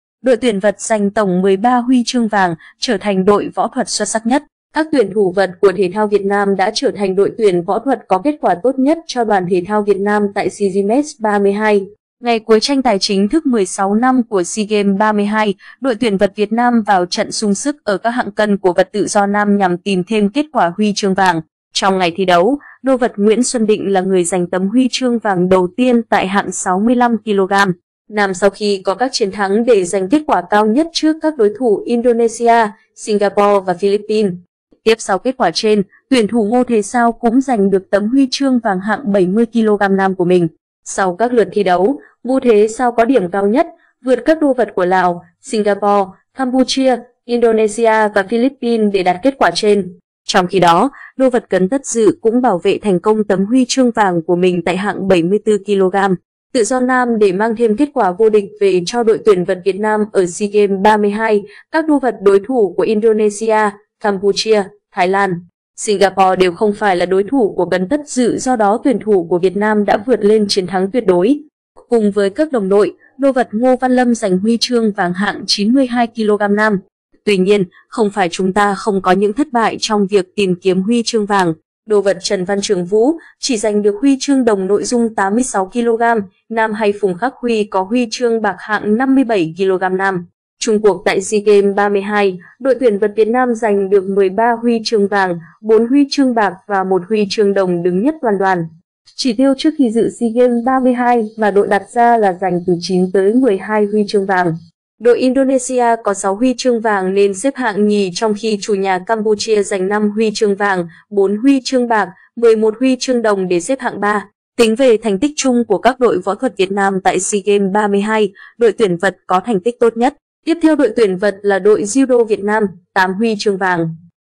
Đội tuyển vật giành tổng 13 HCV, trở thành đội võ thuật xuất sắc nhất
Các tuyển thủ vật của thể thao Việt Nam đã trở thành đội tuyển võ thuật có kết quả tốt nhất cho Đoàn thể thao Việt Nam tại SEA Gmes 32.Nguyễn Xuân Định đã có tấm HCV tại SEA Games 32 cho Đoàn thể thao Việt Nam. Ảnh: DŨNG PHƯƠNG
Trong ngày thi đấu, đô vật Nguyễn Xuân Định là người giành tấm HCV đầu tiên tại hạng 65kg nam sau khi có các chiến thắng để giành kết quả cao nhất trước các đối thủ Indonesia, Singapore và Philippines. Tiếp sau kết quả trên, tuyển thủ Ngô Thế Sao cũng giành được tấm HCV hạng 70kg nam của mình.
Ngày cuối tranh tài chính thức 16-5 của SEA Games 32, đội tuyển vật Việt Nam vào trận sung sức ở các hạng cân của vật tự do nam nhằm tìm thêm kết quả HCV.
Ngô Thế Sao tại SEA Games 32. Ảnh: DŨNG PHƯƠNG
Nguyễn Xuân Định đã có những đòn quyết định để giành HCV. Ảnh: DŨNG PHƯƠNG
Tính về thành tích chung của các đội võ thuật Việt Nam tại SEA Games 32, đội tuyển vật có thành tích tốt nhất. Tiếp theo đội tuyển vật là đội judo Việt Nam (8 HCV).
Sau các lượt thi đấu, Ngô Thế Sao có điểm cao nhất, vượt các đô vật của Lào, Singapore, Campuchia, Indonesia và Philippines để đạt kết quả trên. Trong khi đó, đô vật Cấn Tất Dự cũng bảo vệ thành công tấm HCV của mình tại hạng 74kg tự do nam để mang thêm kết quả vô địch về cho đội tuyển vật Việt Nam ở SEA Games 32. Các đô vật đối thủ của Indonesia, Campuchia, Thái Lan, Singapore đều không phải là đối thủ của Cấn Tất Dự do đó tuyển thủ của Việt Nam đã vượt lên chiến thắng tuyệt đối. Cùng với các đồng đội, đô vật Ngô Văn Lâm giành HCV hạng 92kg nam.
Chung cuộc tại SEA Games 32, đội tuyển vật Việt Nam giành được 13 HCV, 4 HCB và 1 HCĐ đứng nhất toàn đoàn. Chỉ tiêu trước khi dự SEA Games 32 mà đội đặt ra là giành từ 9 tới 12 HCV. Đội Indonesia có 6 HCV nên xếp hạng nhì trong khi chủ nhà Campuchia giành 5 HCV, 4 HCB, 11 HCĐ để xếp hạng ba.
Tuy nhiên, không phải chúng ta không có những thất bại trong việc tìm kiếm HCV. Đô vật Trần Văn Trường Vũ chỉ giành được HCĐ nội dung 86kg nam hay Phùng Khắc Huy có HCB hạng 57kg nam.